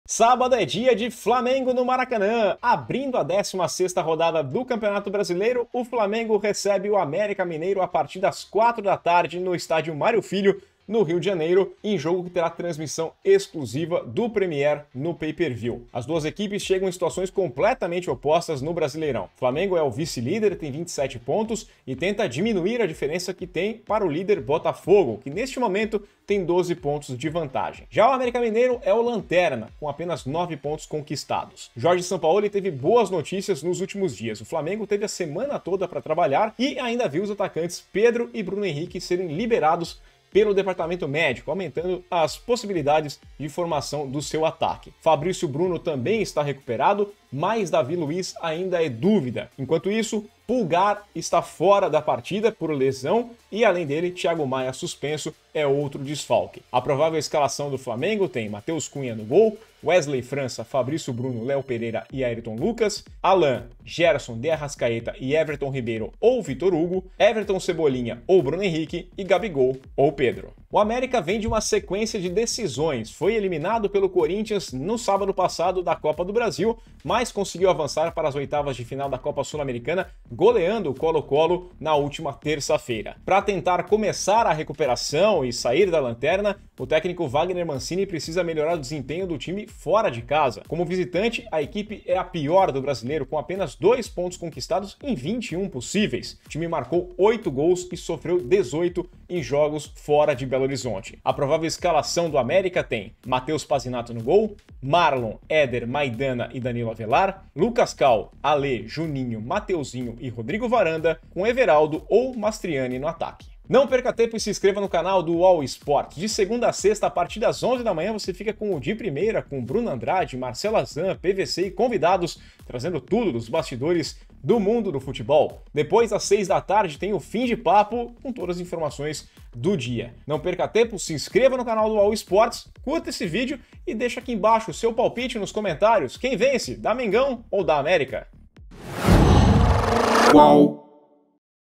FLAMENGO X AMÉRICA-MG: ONDE ASSISTIR AO VIVO, HORÁRIO E ESCALAÇÕES DO JOGO PELO BRASILEIRÃO
Flamengo x América-MG se enfrentam às 16h (horário de Brasília) deste sábado (22), no Maracanã, pela 16ª rodada do Campeonato Brasileiro. A transmissão ao vivo será feita com exclusividade pelo Premiere no pay-per-view. O Placar UOL também acompanha todos os lances em tempo real.

Confira a provável escalação do Flamengo de Jorge Sampaoli, Ayrton Lucas, Pulgar, Gerson, Arrascaeta, Gabigol e Pedro, e a provável escalação do América de Vagner Mancini..